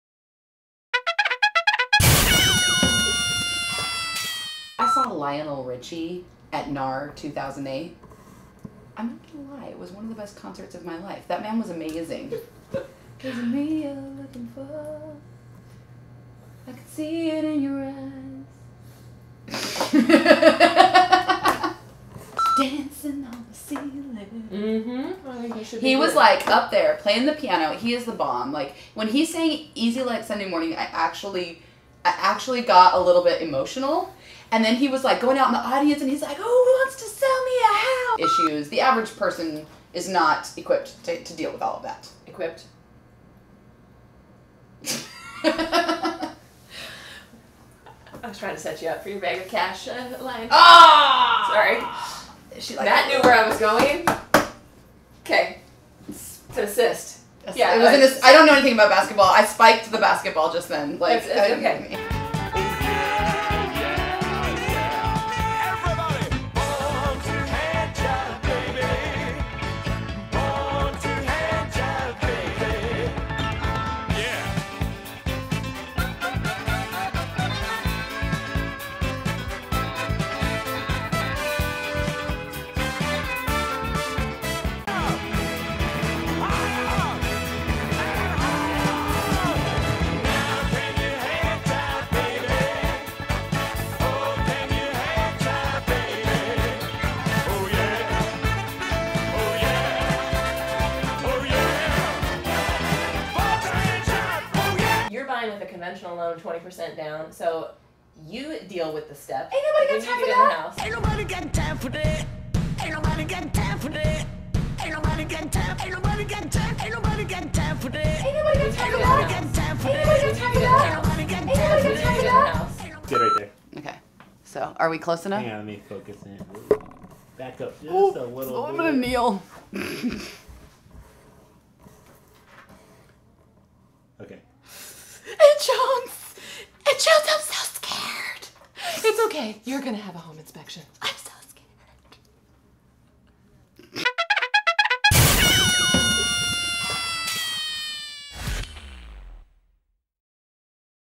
Marguerite & Anne Talk Tacoma Real Estate: The Outtakes, Season Two
Sometimes, when explaining particularly complex real estate topics, there are no words...only hand gestures. (After watching this, I now fear this is all my clients are getting when I'm explaining a contract to them).